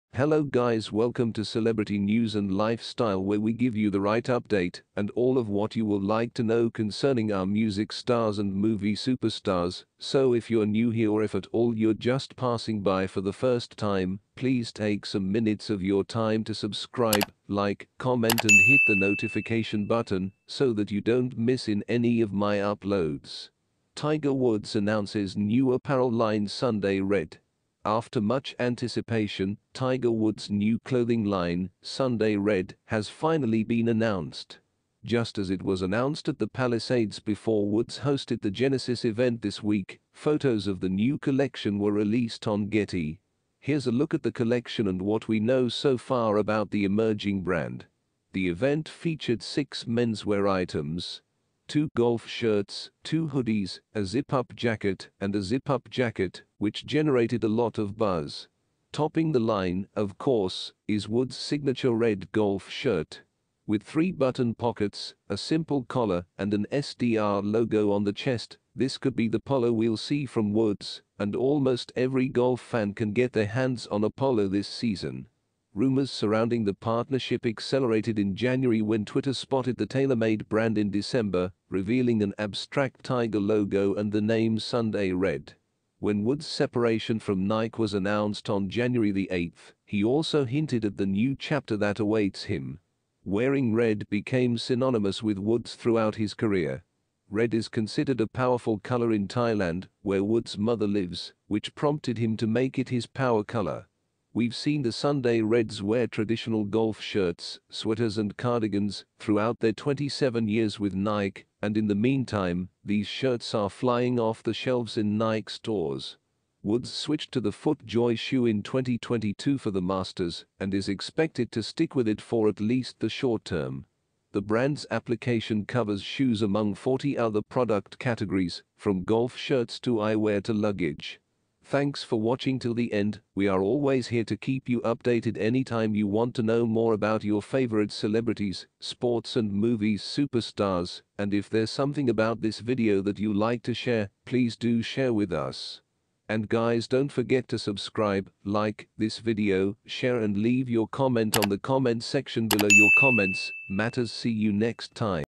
Tiger Woods announces new apparel line Sunday Red
Tiger Woods announces new apparel line Sunday Red. After much anticipation, Tiger Woods' new clothing line, Sunday Red